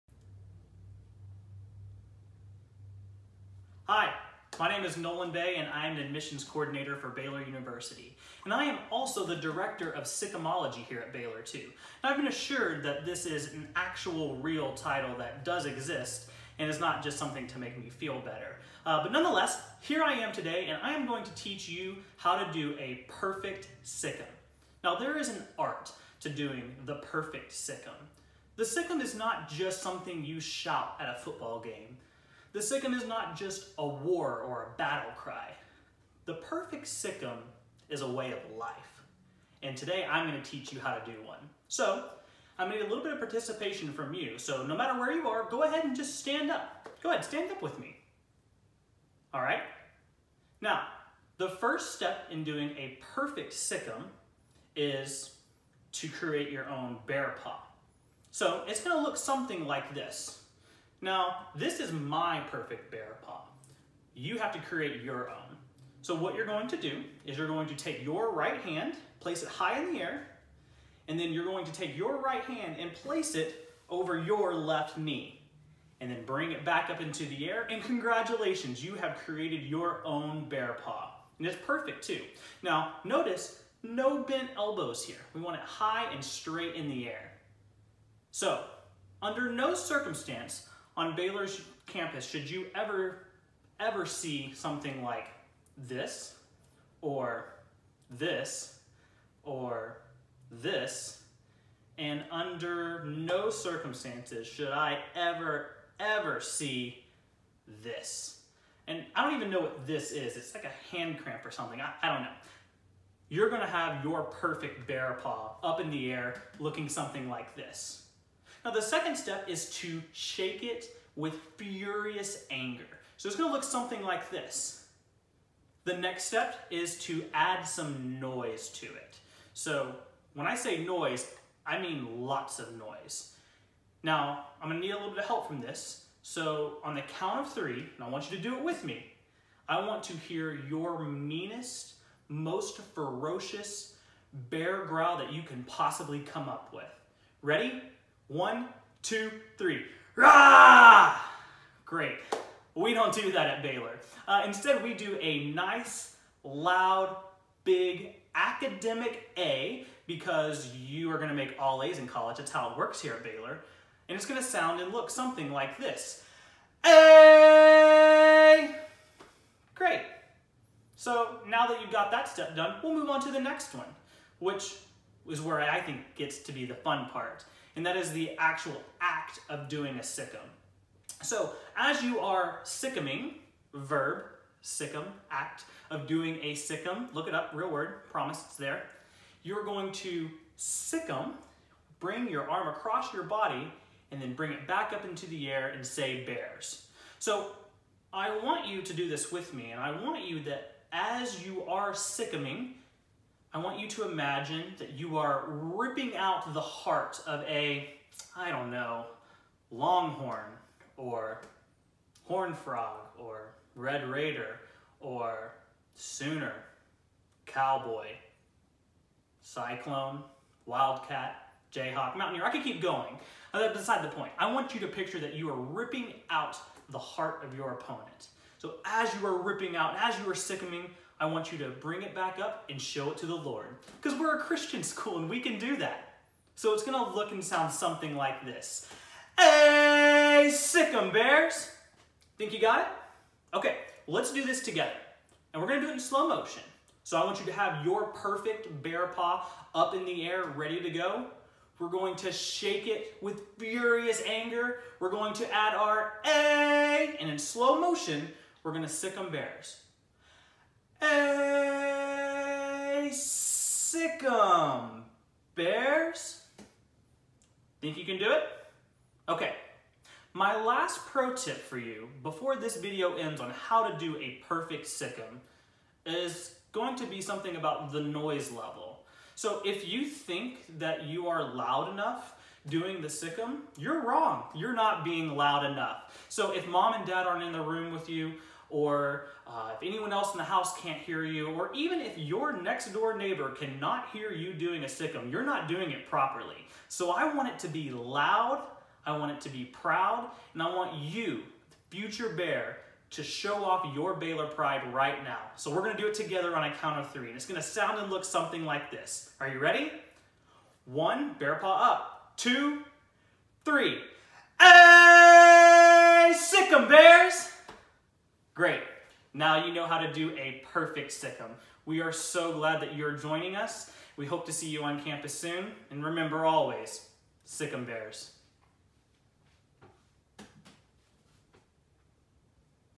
Learn How To Sic ’Em | Baylor University Admissions
Learn the art of the Sic 'Em with Nolan Bay Admissions Coordinator & Director Of Sic’emology (a totally real, valid title).